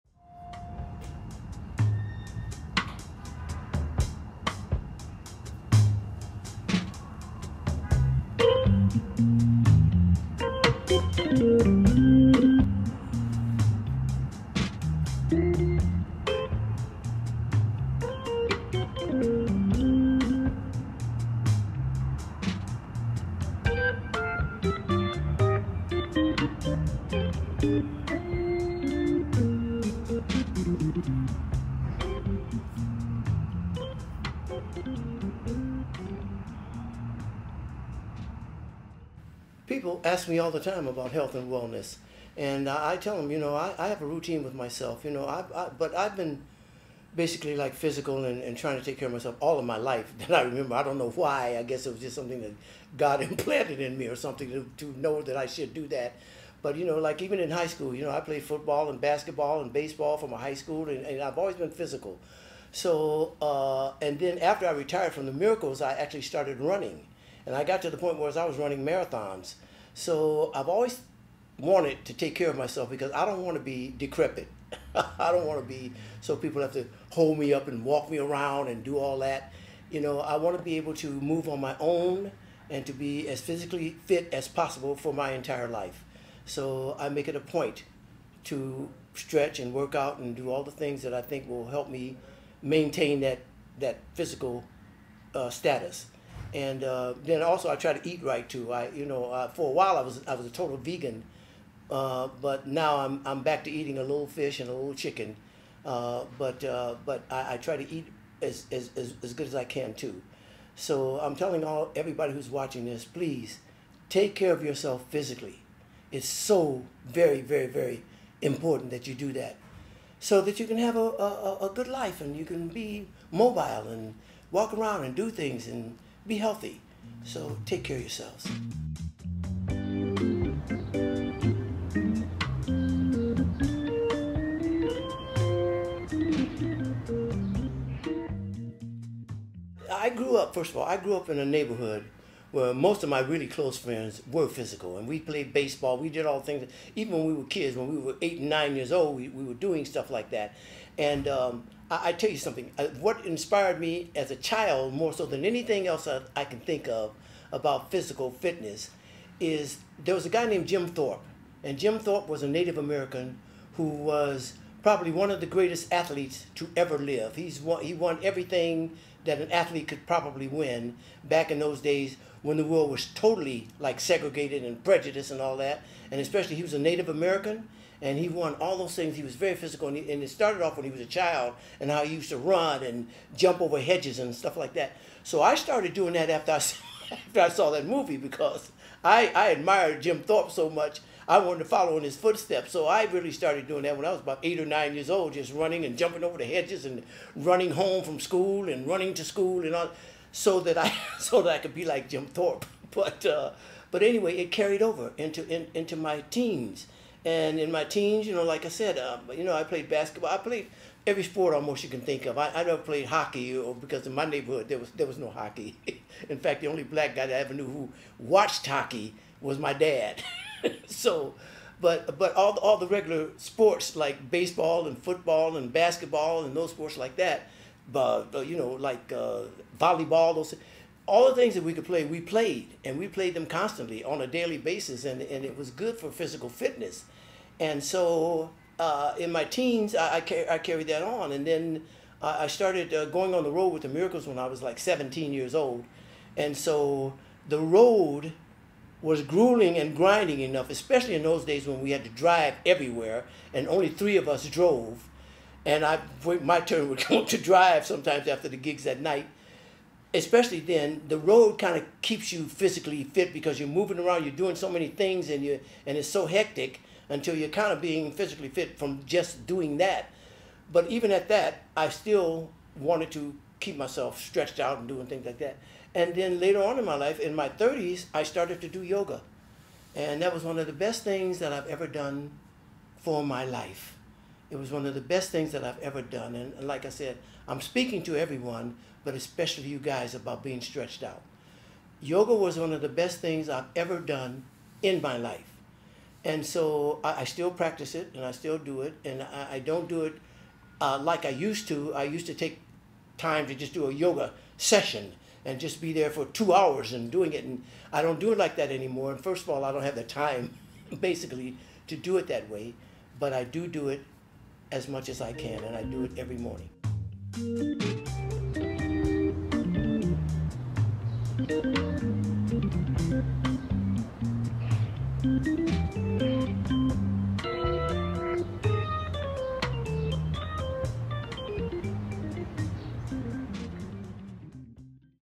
Part One Smokey Robinson Fitness and Health
Every year people all over the world make a "New Year's Resolution" to work out and improve their health. In this four part series, Smokey Robinson shares his resolution to make good health a "lifestyle".  He hopes to inform and inspire people at any age to be more intentional about their own health and wellness, while revealing his own compound benefits of consistent healthy habits over the years.

Shot and edited by: DC Pro
additional edits by: Jaedon Rooney IG: @jaedonnnnn